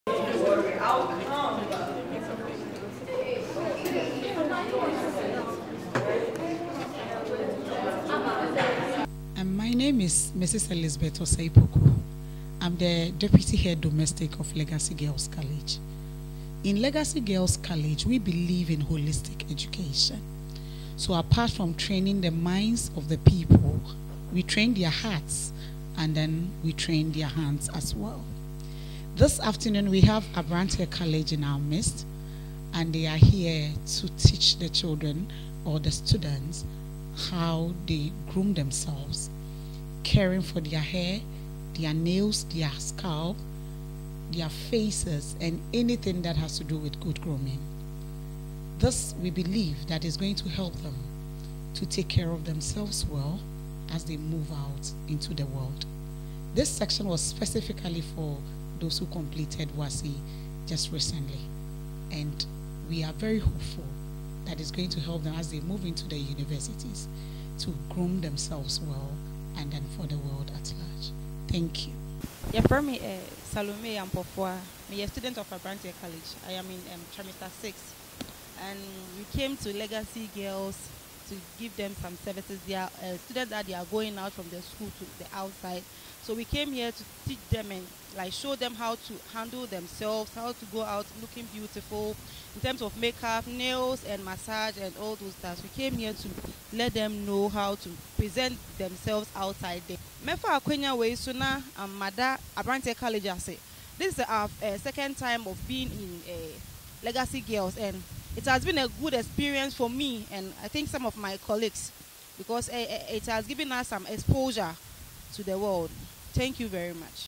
We don't only teach, we also prepare their mind for the future.
Abrantie college, a renowned cosmetology institution in the Greater Accra Region, has as part of their activities for the year, organized a one day practical exercise at Legacy Girls College, in the Greater Accra Region.

The program was to orient the outgoing students of Legacy Girls College on how to groom themselves as they leave the school for their various homes.

In an interview, the Deputy Head Domestic, Mrs. Elizabeth Osei Poku expressed her profound gratitude towards the management of Abrantie college for their kind gesture.

She said, she believes the training will enable the outgoing Students of Legacy Girls College take good care of themselves and broaden their knowledge about beautification and good appearance.